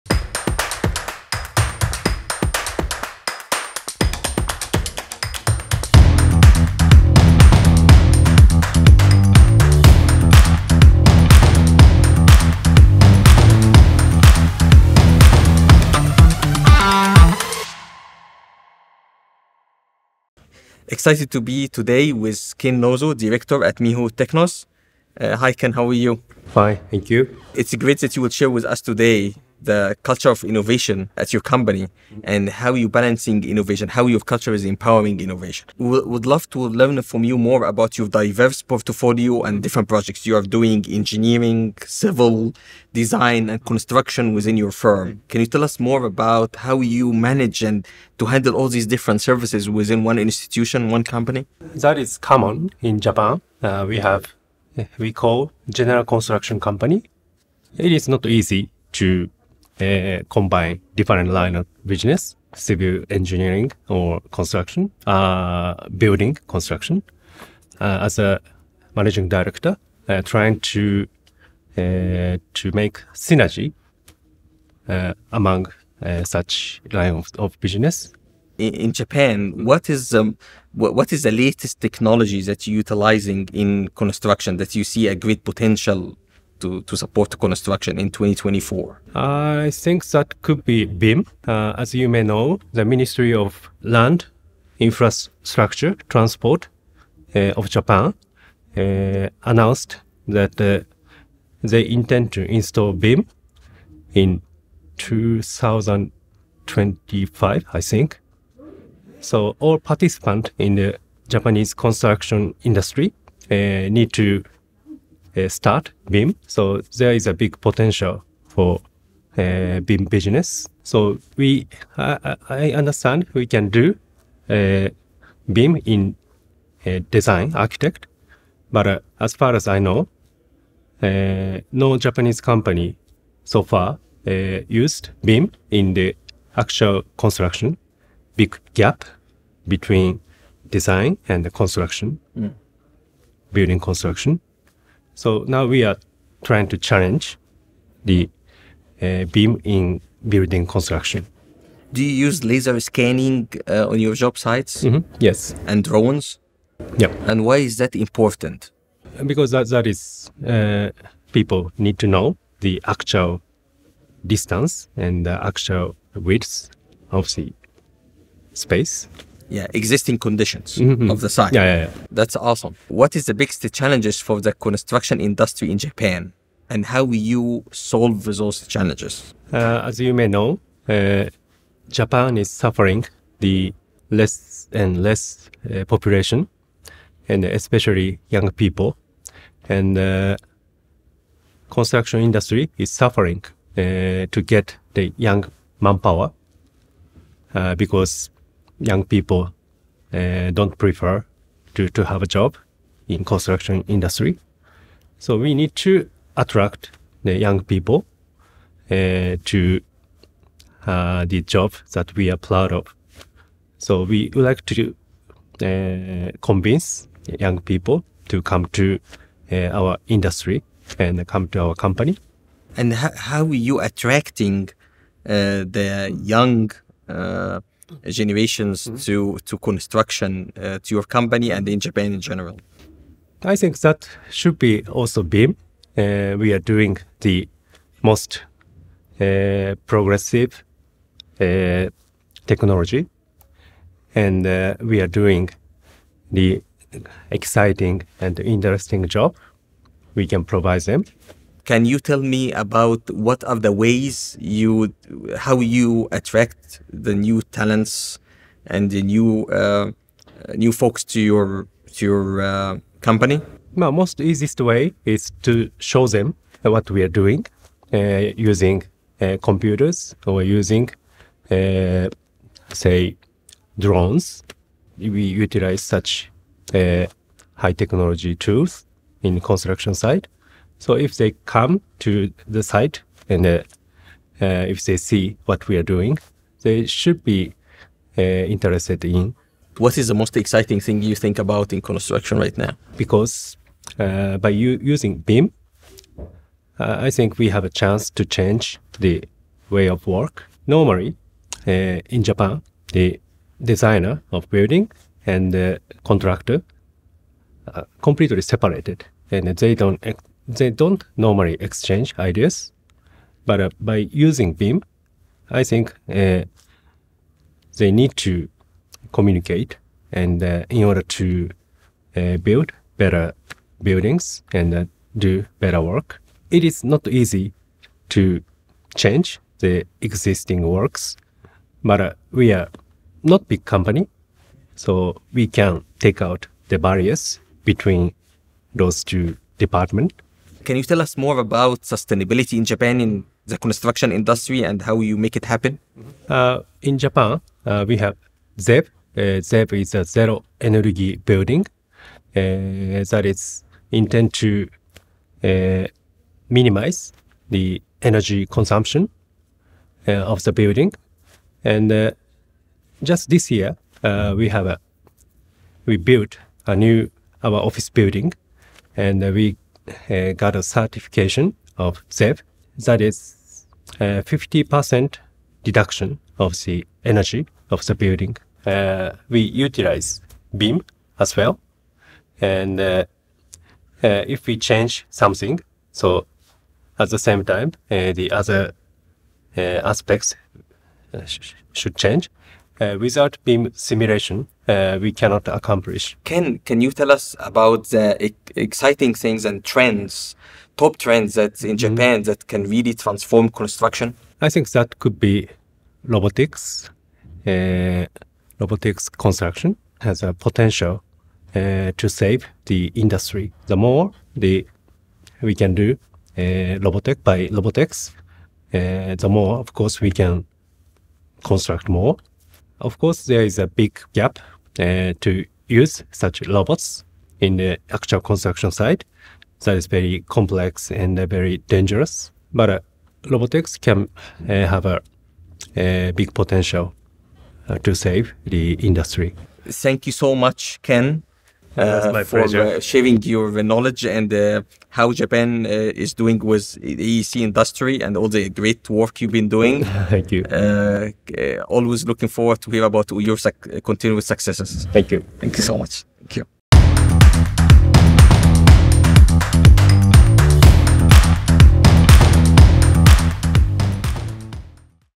The Future of Construction in Japan
Explore the fascinating intersection of tradition and innovation in Japan's construction industry in our latest video with Amr Raafat.

We had the privilege of speaking with Ken Nozu, Managing Director at Miho Technos, who shared insights on how they are embracing technologies like BIM and drones, attracting new talent, and promoting sustainability in their projects.

Don't miss out on this insightful conversation as we uncover the future of construction in Japan.